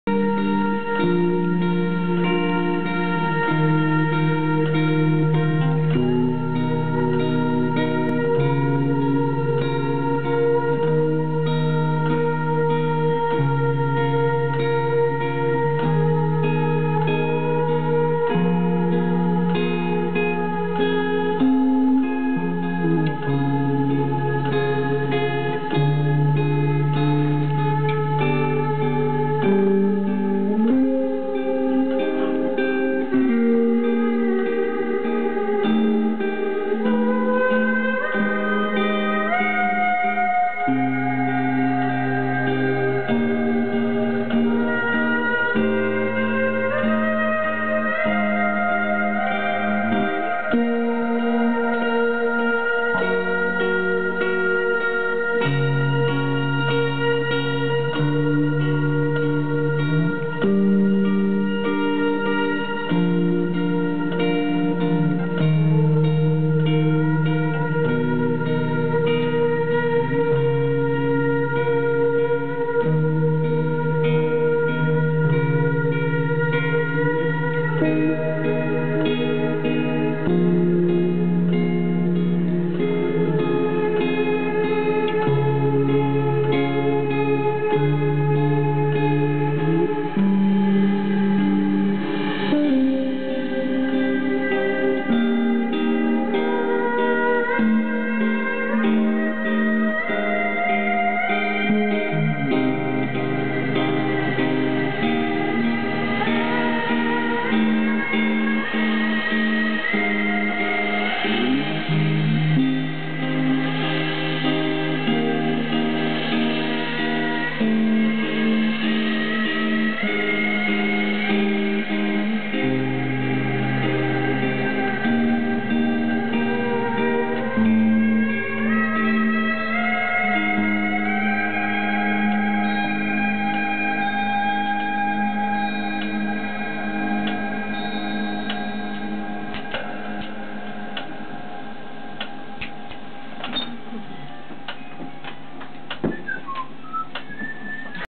Guillaume Perret & Electric Epic IV / VI 2011 NEW ! "guest : Bouygues Telecom"
Guillaume Perret & Electric Epic 11 Juin 2011 au Triton (93)

Guillaume Perret saxophone, effets électroniques et composition
James Mac Gaw guitare
Philippe Bussonnet basse
Yoann Serra batterie